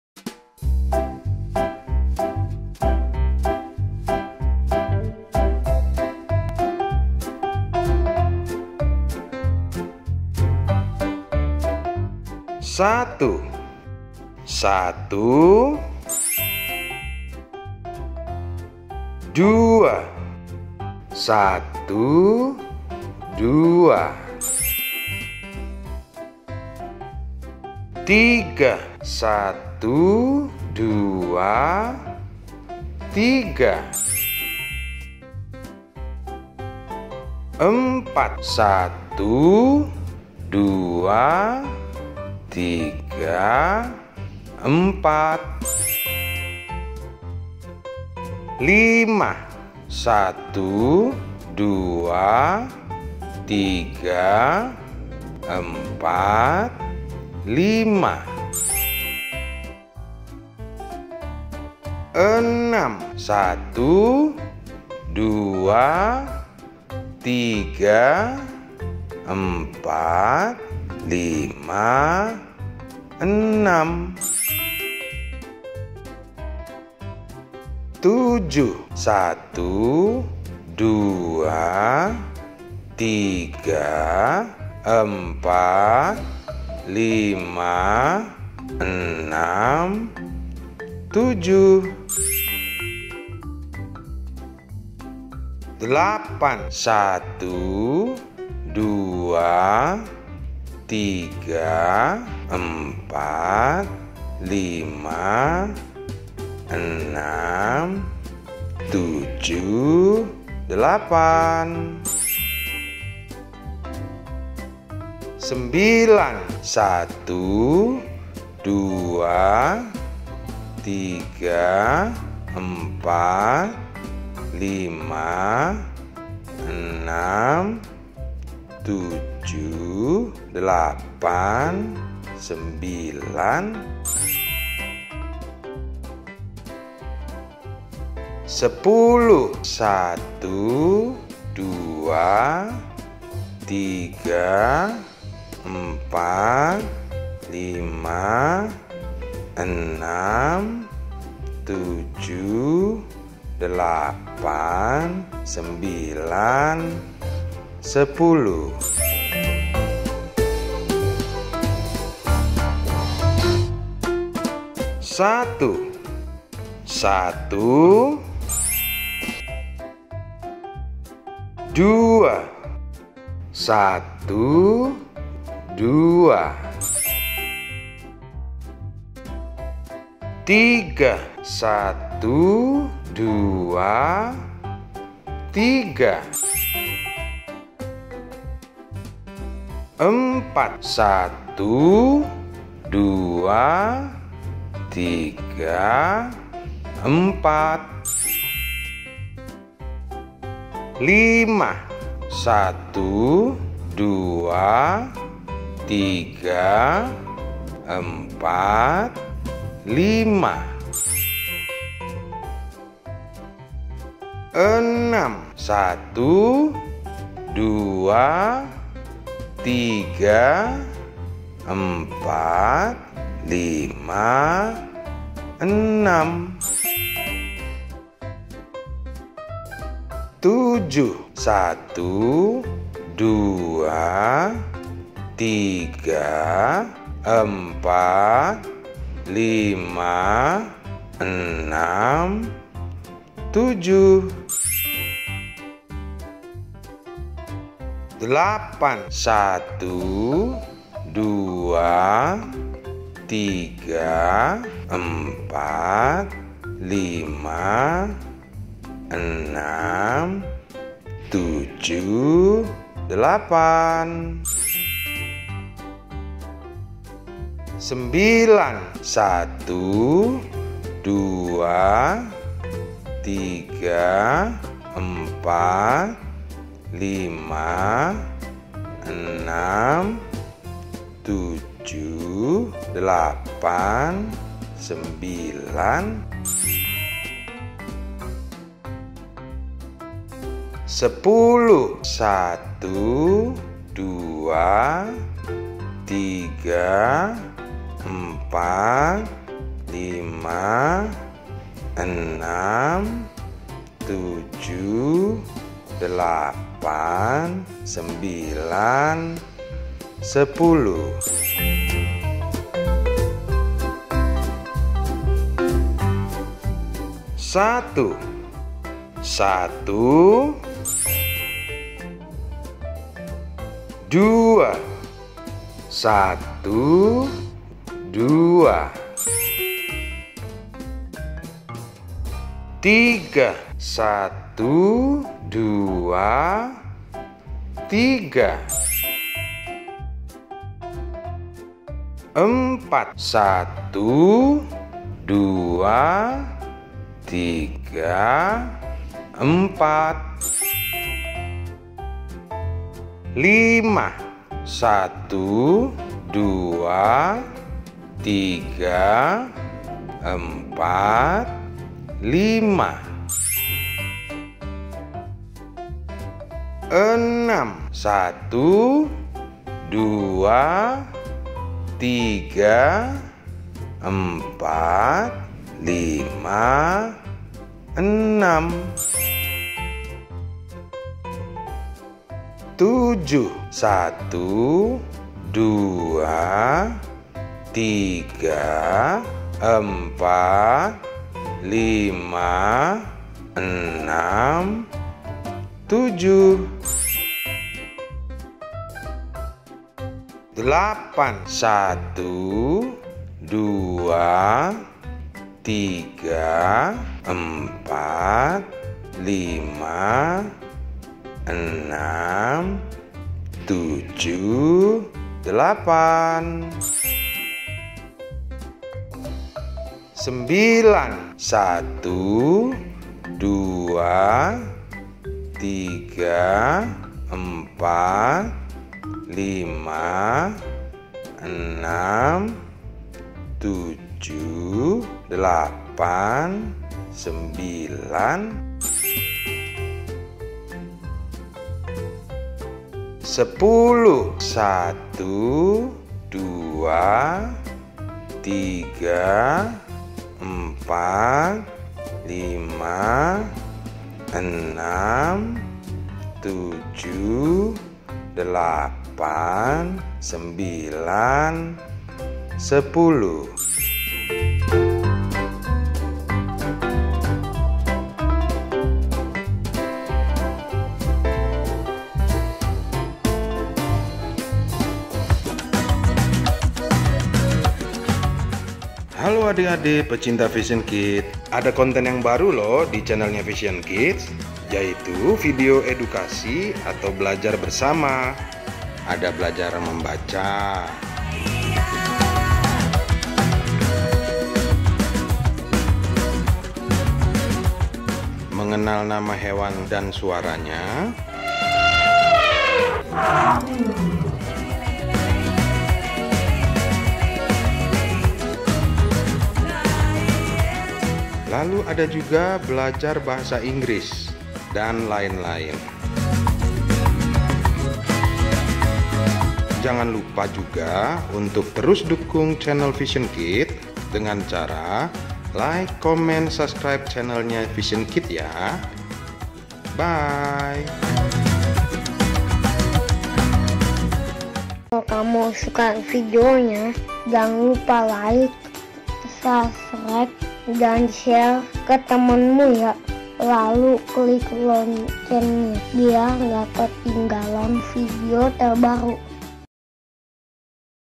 BELAJAR BERHITUNG | UNTUK PAUD & TK
Hallo adik-adik!
Mari kita belajar menghitung angka 1 sampai 10, ikuti baik-baik yaa! semangat belajarnya adik adik...
Kami ada di Instagram!

Moms and Dads, intip konten lainnya :

Terimakasih banyaak

[Indonesia]